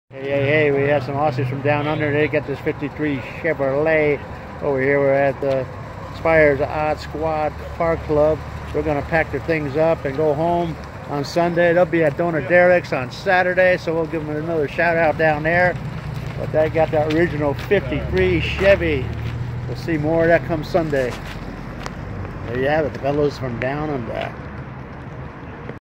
53 Chevy all original Aussie owned!
53 Chevy all original Aussie owned!
One last appearance in a few hours at world famous Donut Derelicts in Huntington Beach, Ca. Hope to see them!